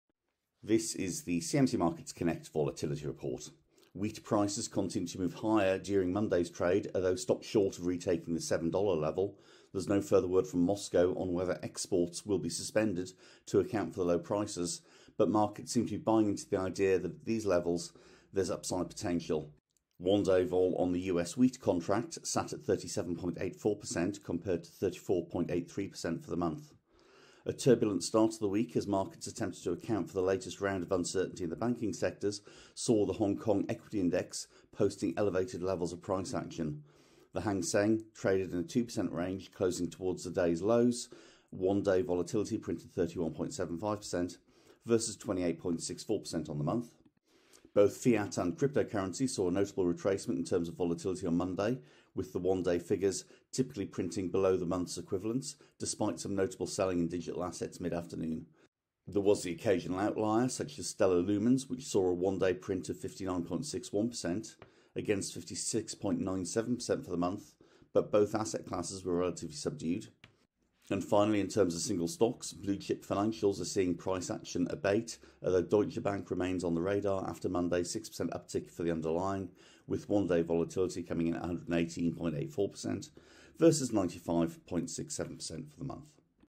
Wheat price volatility advancing higher amidst supply concerns
Current market conditions are shaking up market volatility across a wide range of instruments. See what is trending in terms of volatility and where the opportunities are, as highlighted by our Quant and Trading desk.

For more market insights, volatility data and trends in your inbox, visit our insights hub